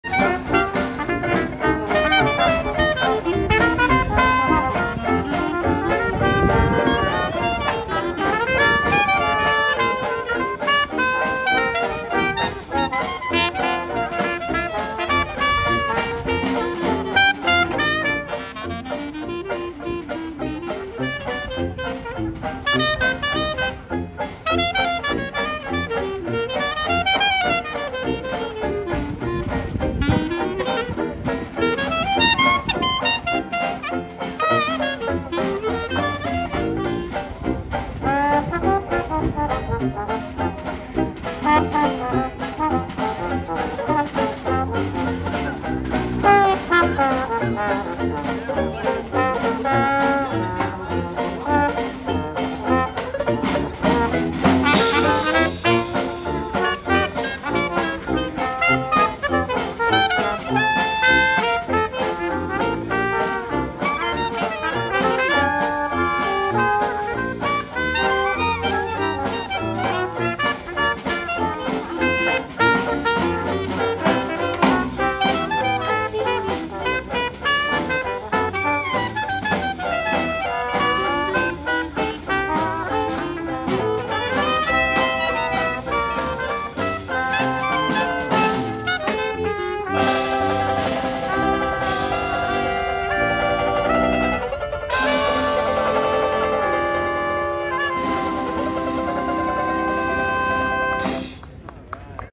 Disneyland January 16,2023 Main Street Band
Main Street Band performance in front of Great Moments with Mr. Lincoln.